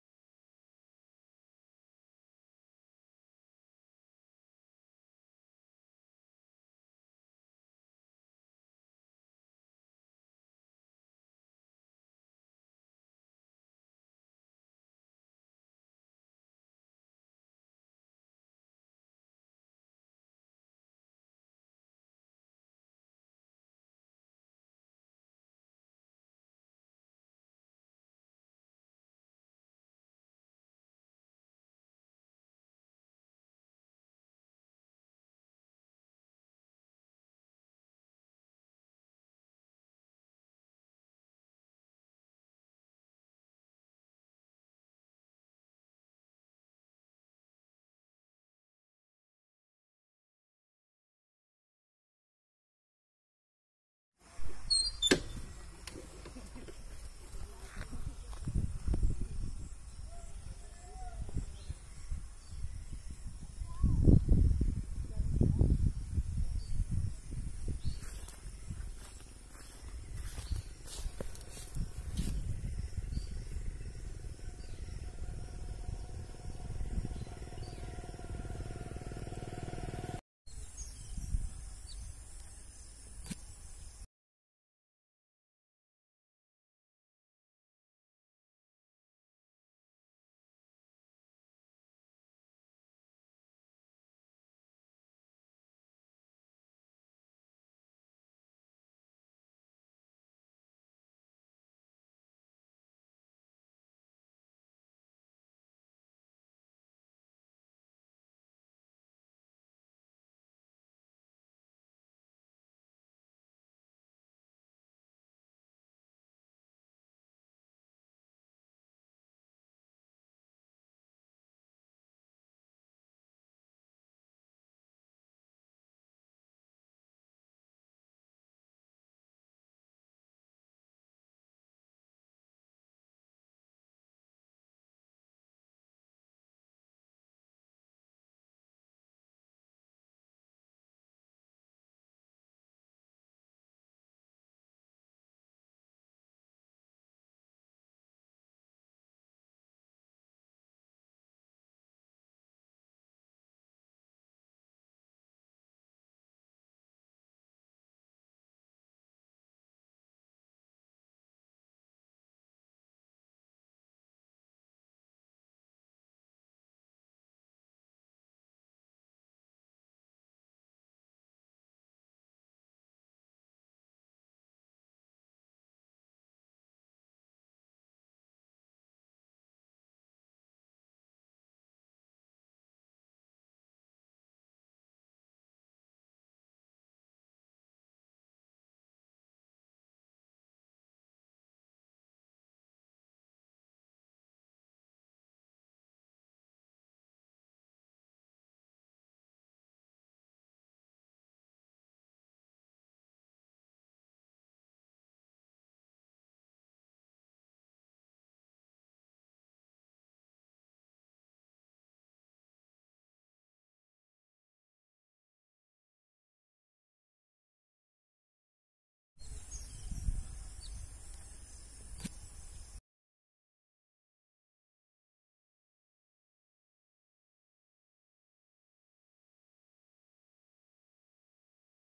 1 Acre 10 Gunta Farm Land Sale//subramanya m r // subscribe me //
10 Gunta Extra Free Land
Ready to Registration
Clear Documents
1 Running Borewell With Good Water Source
90 Kilometers from Bangalore
NH 209 Kanakapura Road
Areka Sipplings
Coconut 🌲
Teek 🌲
Rs_50 Lakhs For Total LAND
Tar Road Facing
Total Fencing
for more details contact my number

This is subramanya m r
Please Share Like & Support Me
General Properties